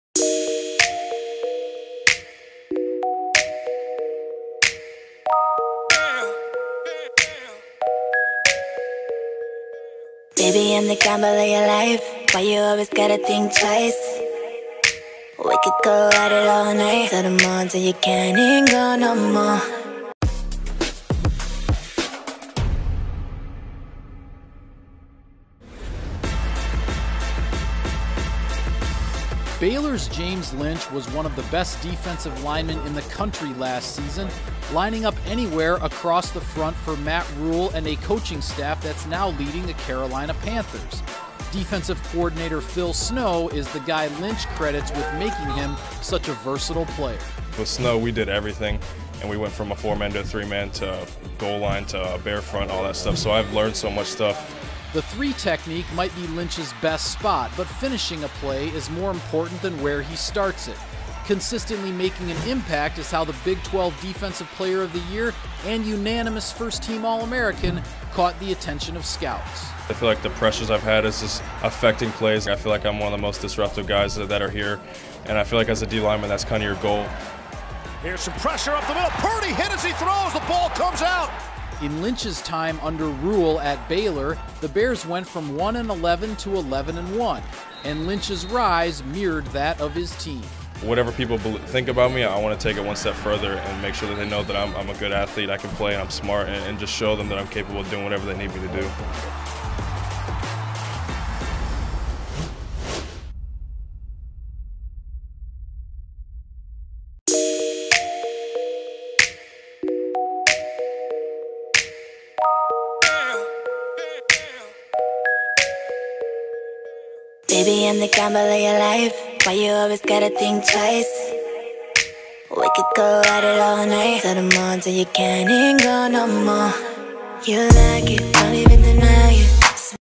Prospect Primer: DL James Lynch
DL James Lynch posted 13½ sacks among 19½ tackles for loss in 2019.